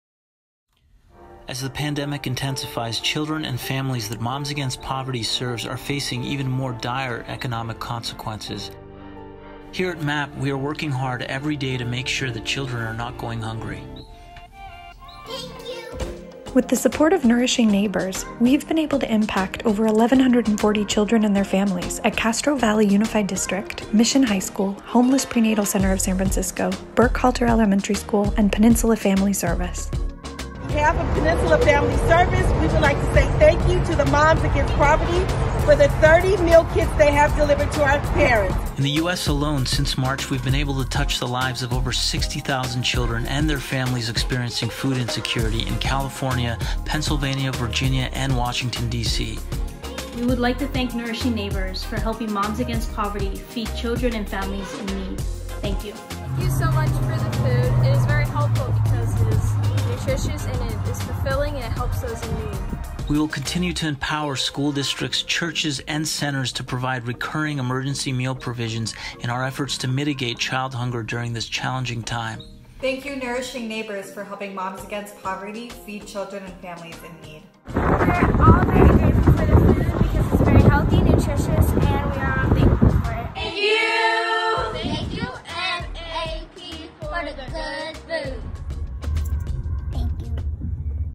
US Hunger Relief Efforts & Nourishing Neighbors Grant
MAP's Nourishing Neighbors grant from Albertsons, and our continued impact on hunger relief.
Nourishing Neighbors raises awareness, engages volunteers and raises funds to support innovative and effective programs throughout the country focused on ensuring every child in America has access to a healthy breakfast.